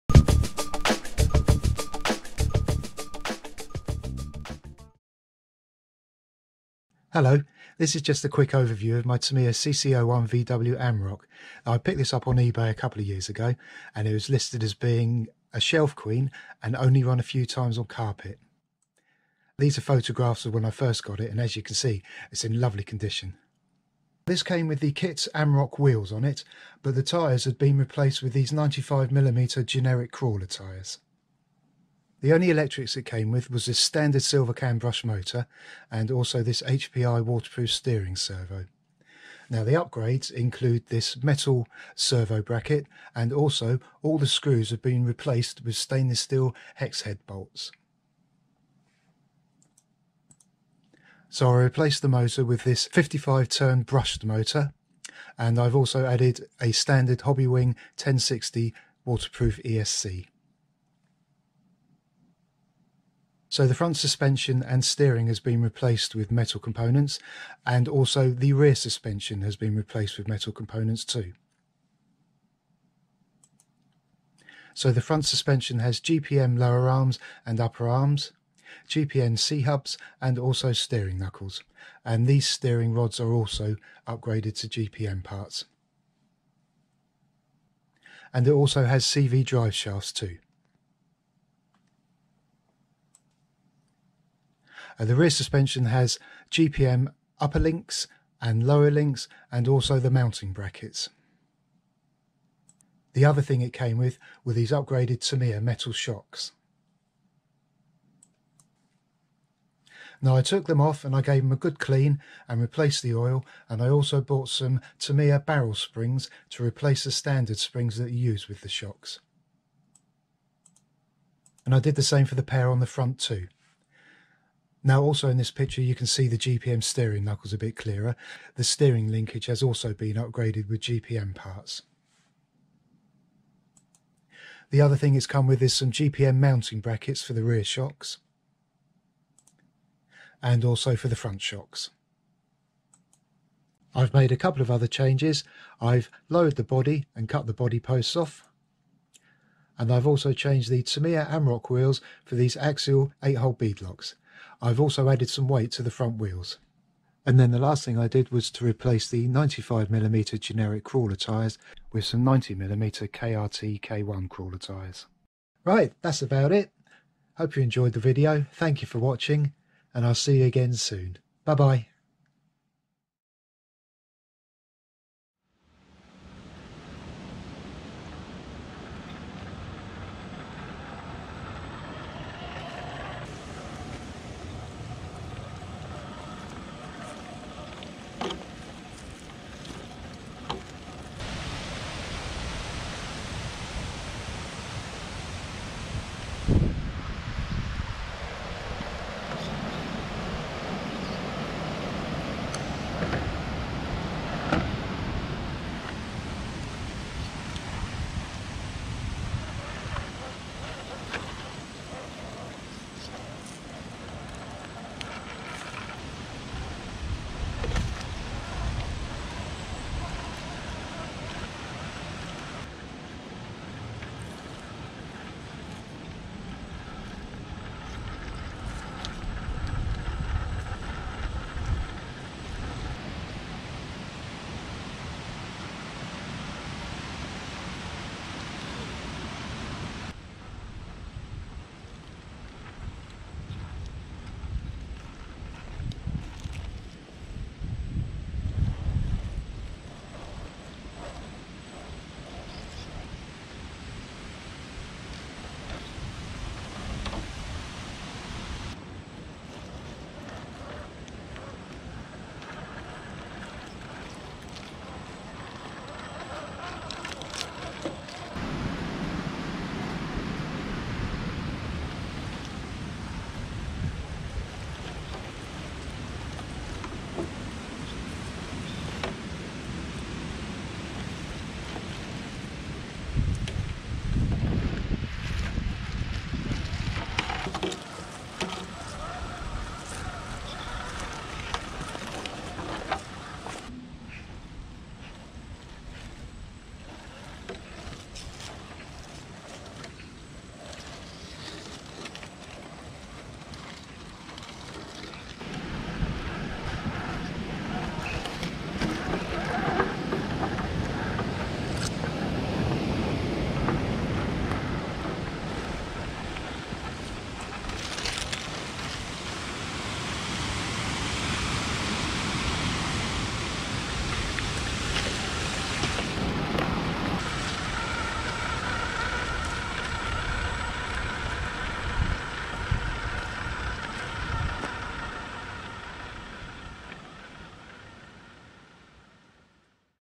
Tamiya CC-01 VW Amarok Upgrades & Run
Tamiya 58616 CC-01 VW Amarok

Spec:-
Hobbytech 55T 540 Brushed Motor (from a Hobbytech CRX Survival Kit)
Hobbywing Quicrun 1060 60A Waterproof Brushed ESC HW30120201
Hpi SF-10W 4.5kg Waterproof Servo Hpi-104105 (Discontinued)
Tamiya 54541 CC-01 OP.1541 CC01 Aluminum Oil Damper Kit
Tamiya 54666 CC-01 Barrel Spring Set
GPM Racing CC054/M3-S-BEBK Aluminum Alloy Upper Arm
GPM Racing CC054-S Aluminum Front Upper Suspension Arm Set
GPM Racing CC055-S Aluminum Alloy Front Lower Suspension Arms
GPM Racing CC020-S Aluminum C-Hubs
GPM Racing TA3021-S Aluminum Front Knuckles
GPM Racing SCC245-BK Steel Front CVD Shafts
GPM Racing CC070F/M-S Aluminum Front Damper Top Mounts
GPM Racing CC070R/M-S Aluminum Rear Damper Top Mounts
GPM Racing CC161-S-BEBK Aluminum Alloy Rear Adjustable Suspension Links
GPM Racing CC030M-S Aluminum Alloy Rear Damper Mounts
GPM Racing CC024-S Aluminum Servo Mount
GPM Racing CC048-B Aluminum Steering Assembly Set
Full Metal Ball Bearing Set
Stainless Steel Hex Head Hardware
Axial 1.9 8 Hole Beadlock Wheels Chrome AX8088 (Weights in Fronts)
KRT K1 90mm Diameter Crawler Tyres BRK623011 (Discontinued)